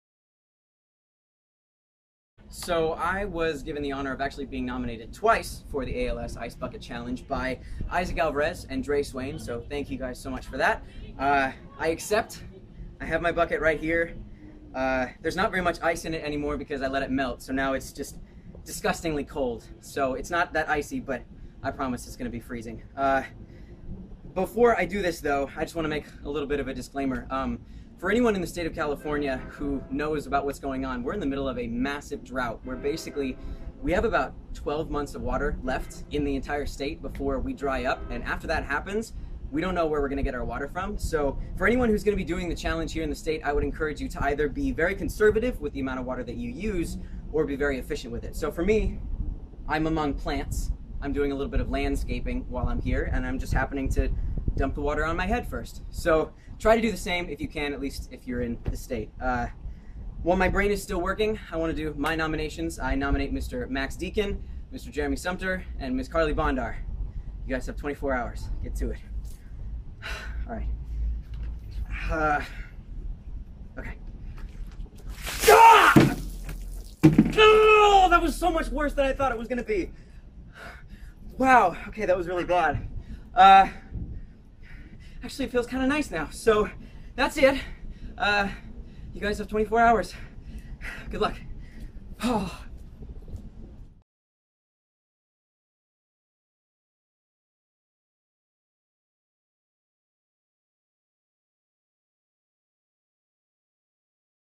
Nathan Kress | SHIRTLESS
Nathan Kress: "Huge thanks to Dre Swain and Isaac Alvarez for nominating me for the ALS Ice Bucket Challenge! In turn, I nominate my Into the Storm costars, Max Deacon and Jeremy Sumpter, as well as Carly Bondar, who will be starting research on ALS in the coming semester! Your donations make that kind of work possible, so please join me in supporting ALS research by donating right now!"